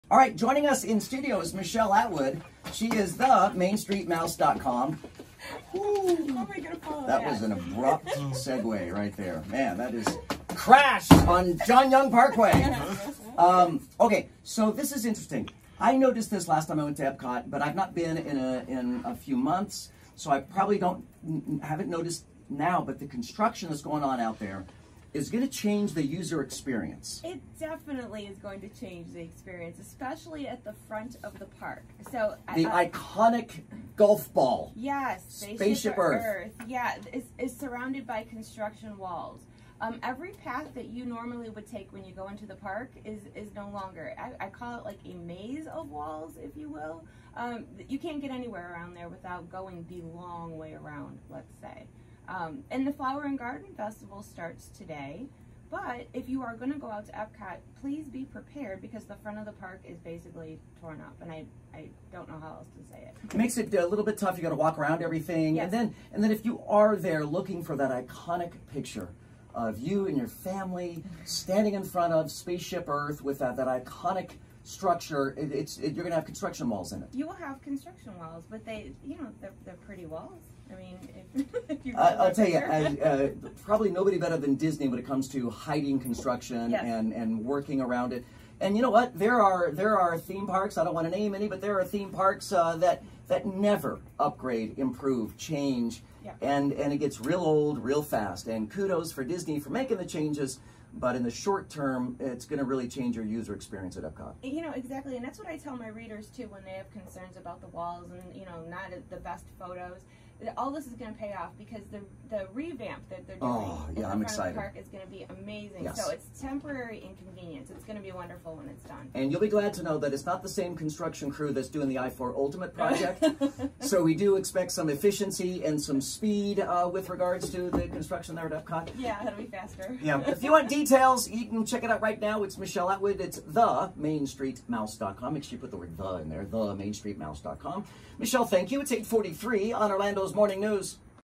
Talking Epcot with Joe Kelley on 96.5 WDBO
We had a fun time this morning on the radio talking about multiple things, some a bit silly. Here’s a clip regarding Epcot!

Follow The Main Street Mouse on Facebook and Instagram at themainstreetmouse!